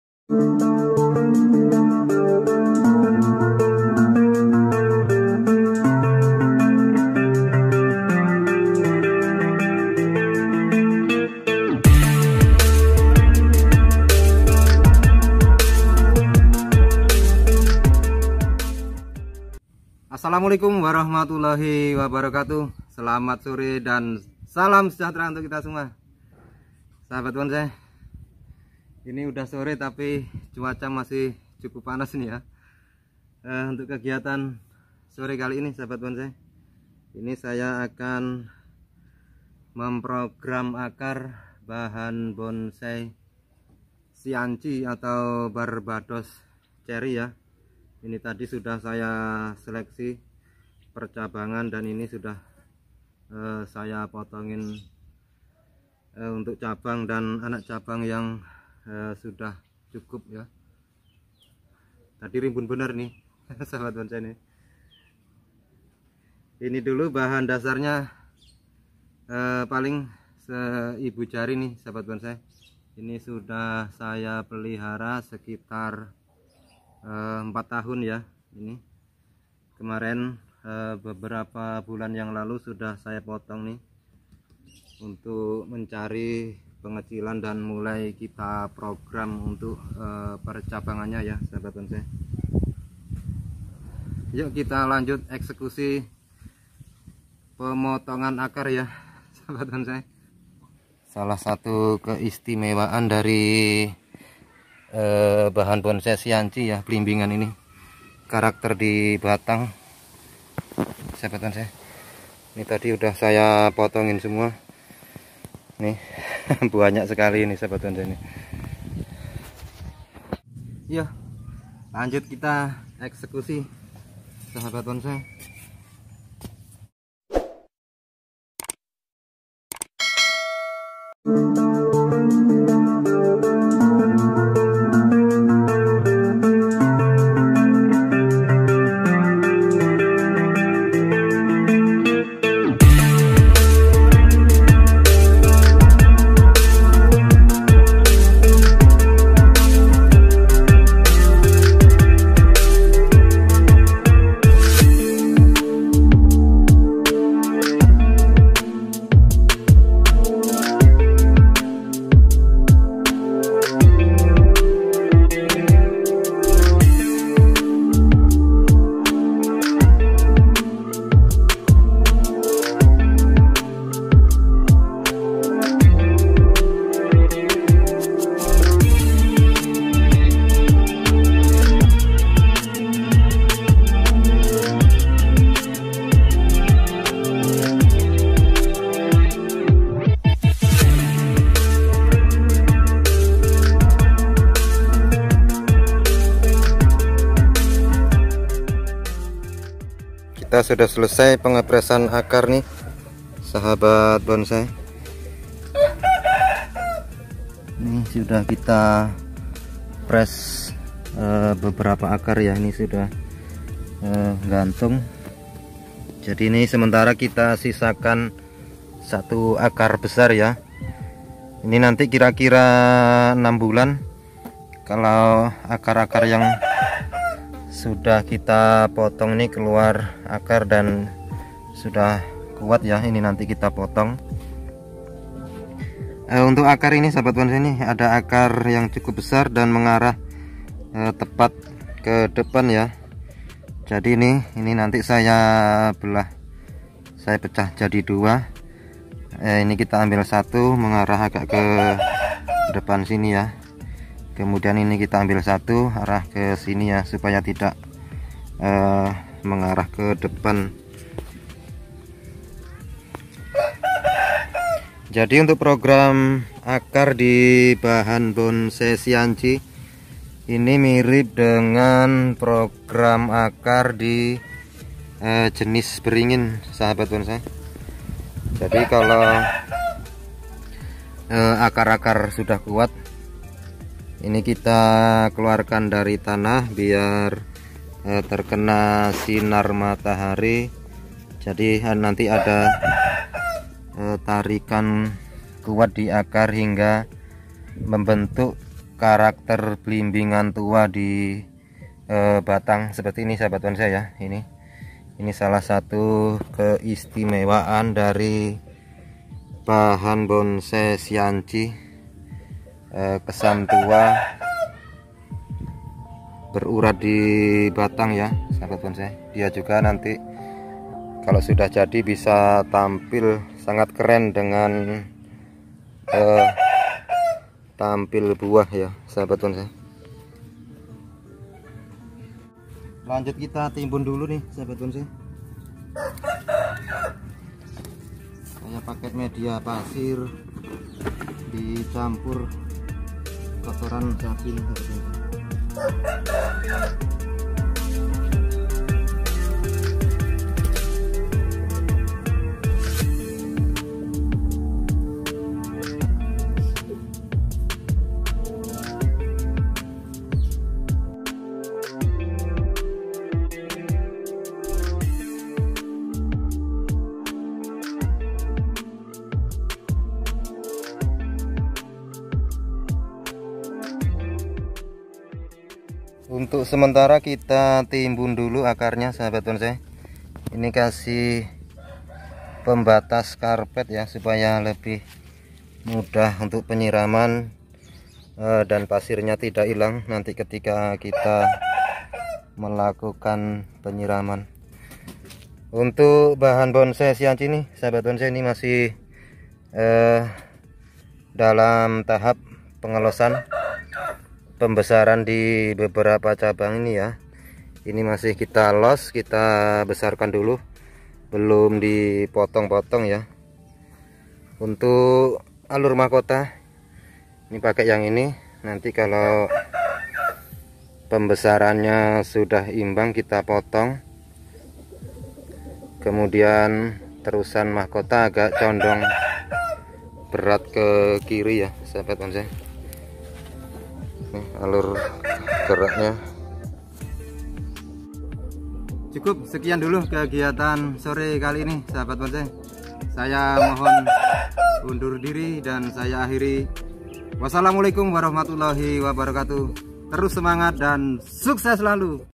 bonsai sianci_barbados cherry_program akar dan perawatan
aktifitas sore ini kita melakukan seleksi cabang dan pengepresan akar di bahan bonsai sianci(barbados cherry) yang berusia sekitar empat tahun dari bahan cangkok sebesar ibu jari orang dewasa.
untuk proses selanjutnya masih dalam tahap pengelosan pembesaran di cabang dan anak cabang.

salam santun:winarno(Bopo Bonsai Art)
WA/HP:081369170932
Domisili:Bandar jaya timur,terbanggi besar
Lampung tengah,Lampung,Indonesia.